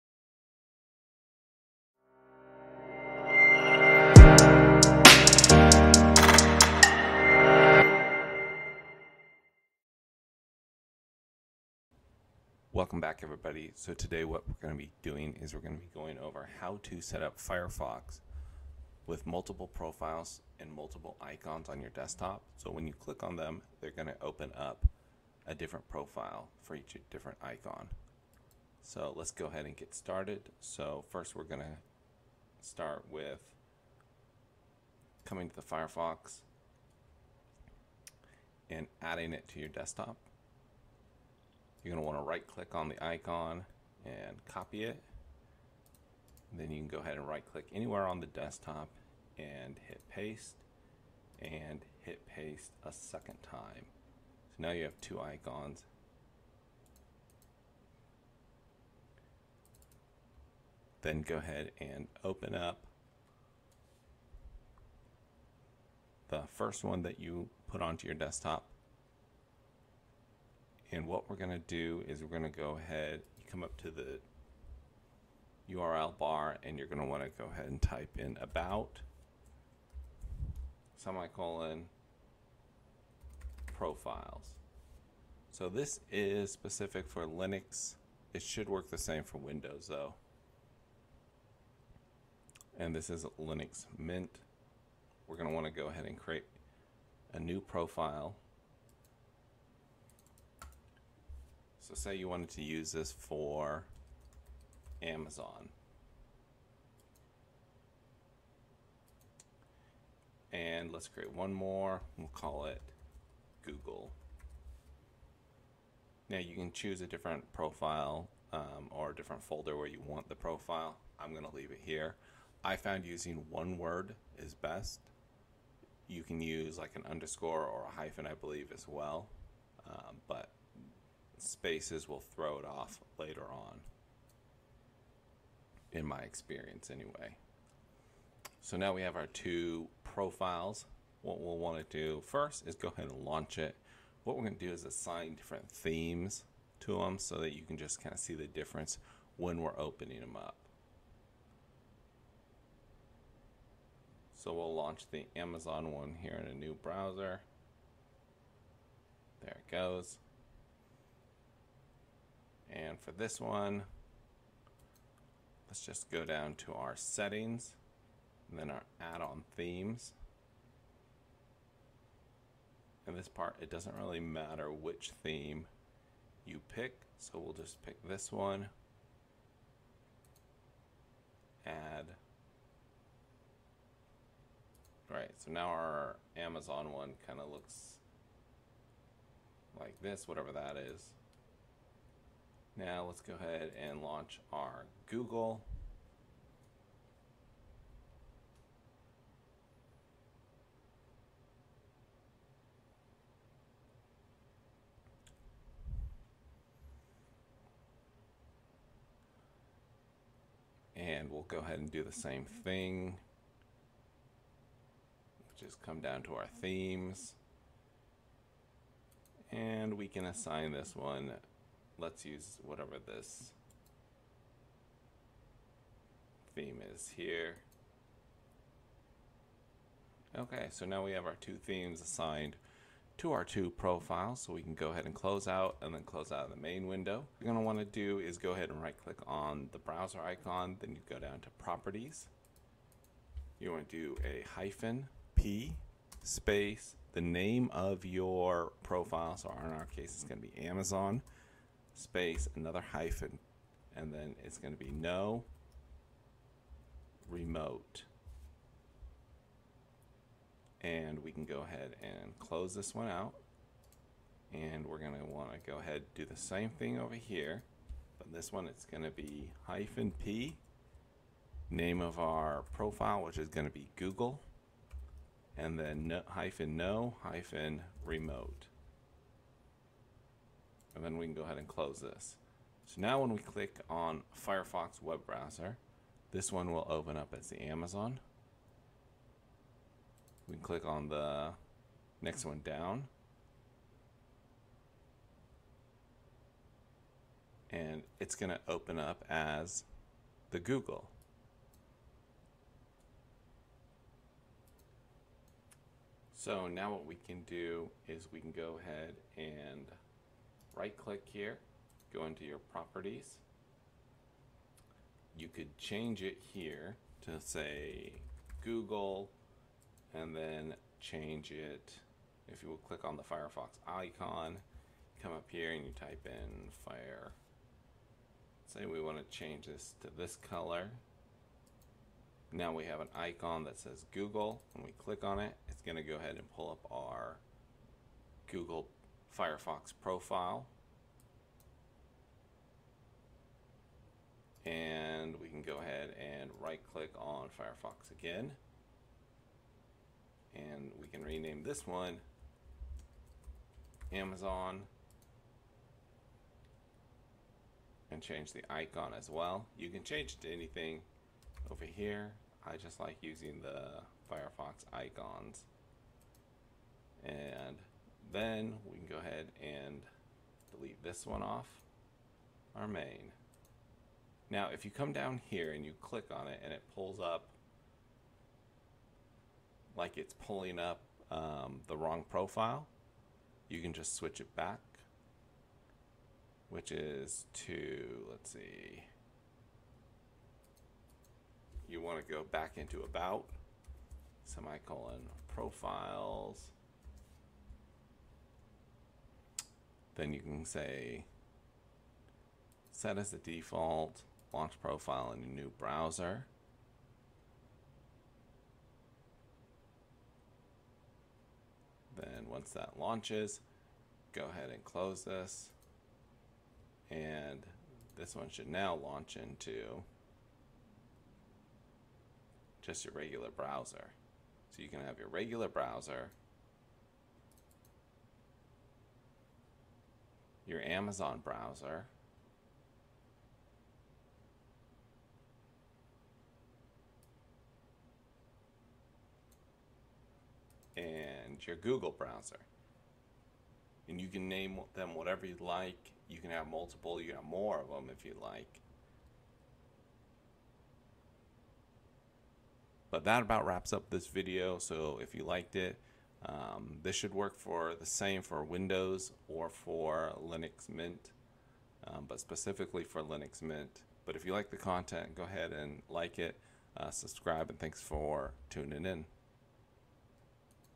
How to Setup Firefox icon profiles on Linux Mint
How to have different icons for Firefox open up different Firefox profiles.  Works best when you have the profiles just one word or separated by hyphens.

Open just profile selector use -p
Open a specific profile -p (Profile name) -no-remote